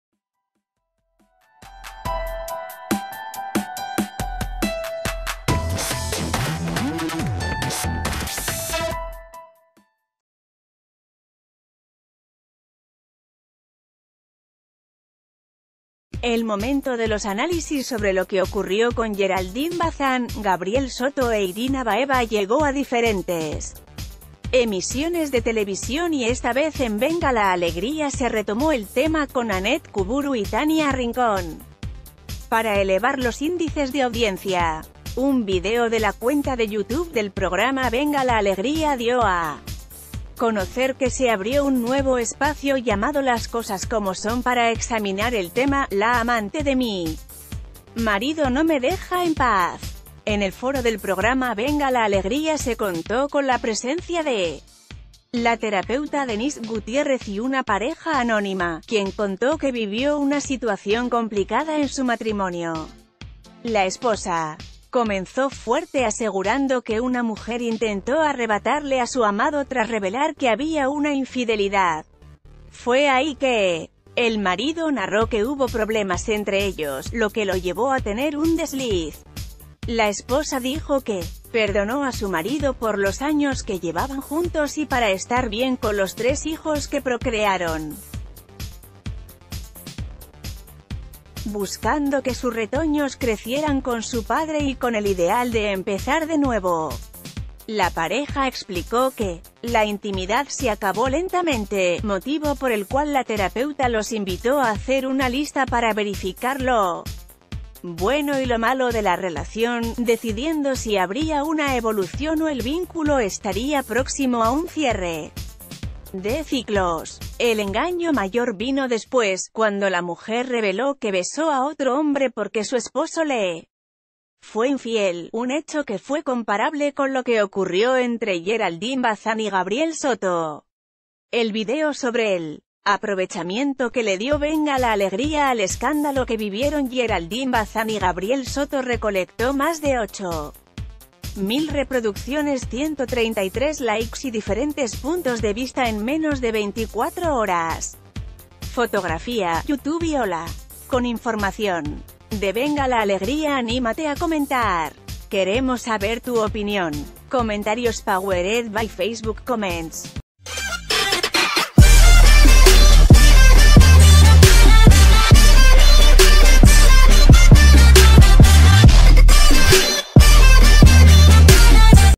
VLA aprovecha escándalo de Geraldine y Gabriel para ganar rating de forma ruin
El momento de los análisis sobre lo que ocurrió con Geraldine Bazán, Gabriel Soto e Irina Baeva llegó a diferentes emisiones de televisión y esta vez en Venga la Alegría se retomó el tema con Anette Cuburu y Tania Rincón para elevar los índices de audiencia. Un video de la cuenta de YouTube del prog...